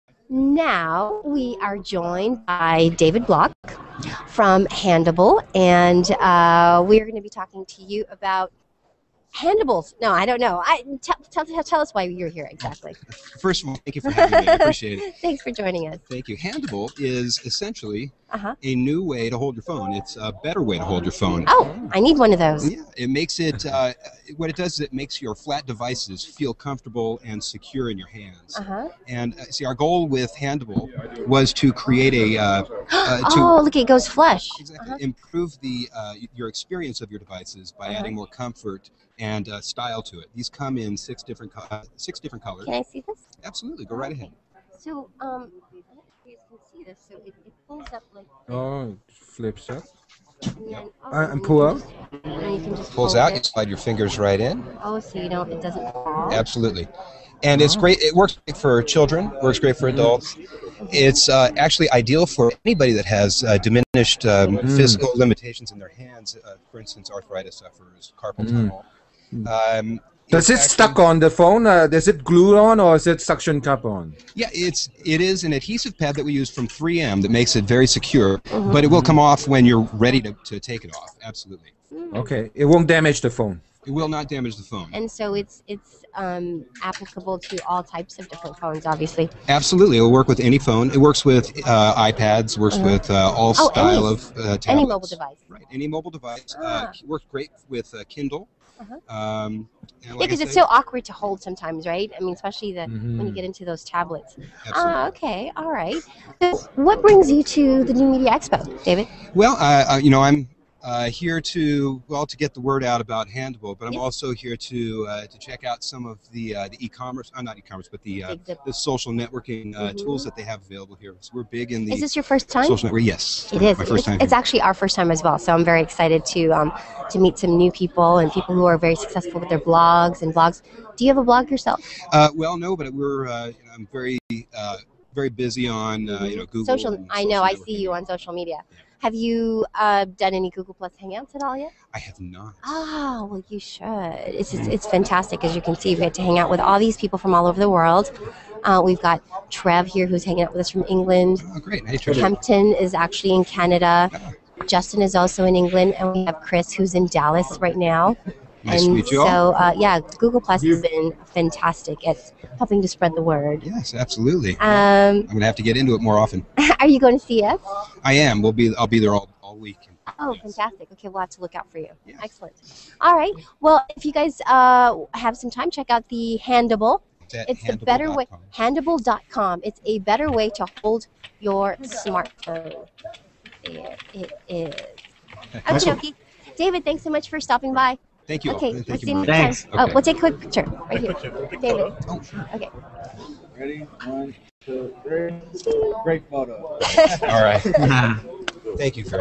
David Block - The Hand Able
MommyLovesTech interviews David Block from Handable